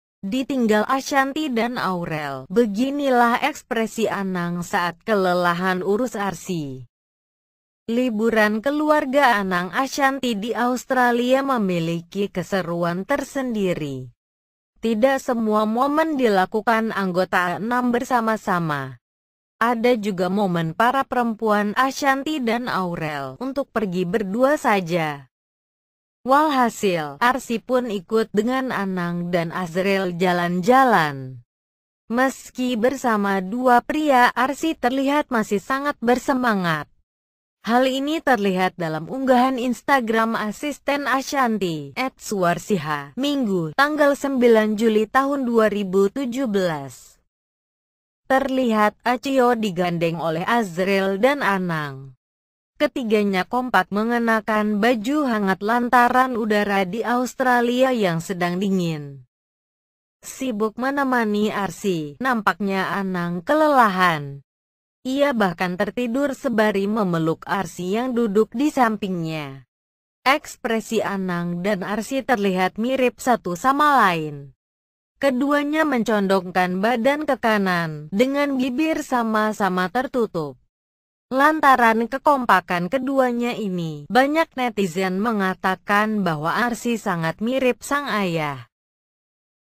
Tertidur sembari memeluk Arsy, Eksperi Anang Banjir pujian HOT Dedy
Ditinggal Ashanty dan Aurel, Beginilah Ekspresi Anang Saat Kelelahan Urus Arsy

Liburan keluarga Anang Ashanty di Australia memiliki keseruan tersendiri.
Tidak semua momen dilakukan anggota A6 bersama-sama.

Ada juga momen para perempuan, Ashanty dan Aurel, untuk pergi berdua saja.
Walhasil, Arsy pun ikut dengan Anang dan Azriel jalan-jalan.

Meski bersama dua pria, Arsy terlihat masih sangat bersemangat.
Hal ini terlihat dalam unggahan Instagram asisten Ashanty, @suwarsiha, Minggu (9/7/2017).

Terlihat Acio digandeng oleh Azriel dan Anang.
Ketiganya kompak mengenakan baju hangat lantaran udara di Australia yang sedang dingin.
Sibuk menemani Arsy, nampaknya Anang kelelahan.

Ia bahkan tertidur sebari memeluk Arsy yang duduk di sampingnya.
Ekspresi Anang dan Arsy terlihat mirip satu sama lain.
Keduanya mencondongkan badan ke kanan, dengan bibir sama-sama tertutup.

Lantaran kekompakan keduanya ini, banyak netizen mengatakan bahwa Arsy sangat mirip sang ayah.